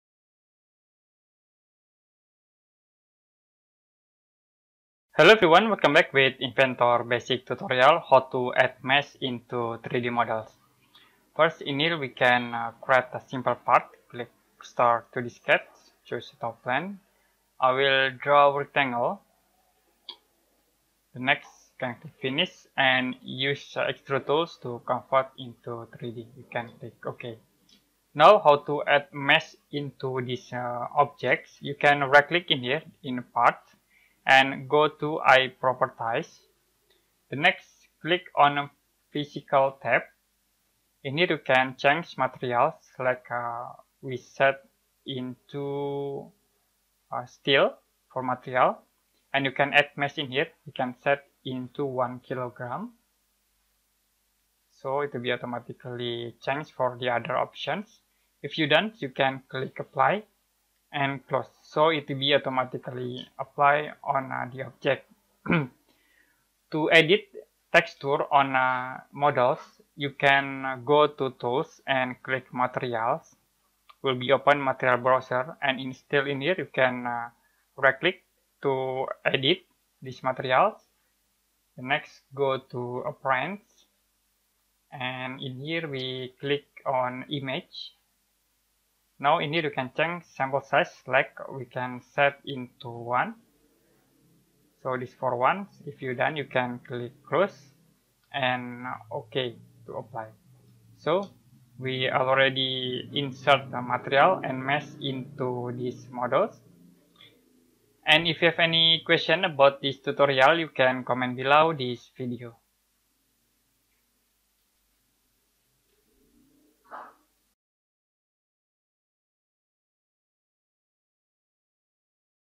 Inventor How To Add Mass On Object
Learn autodesk inventor tips and trick how to add mas on component fast and easy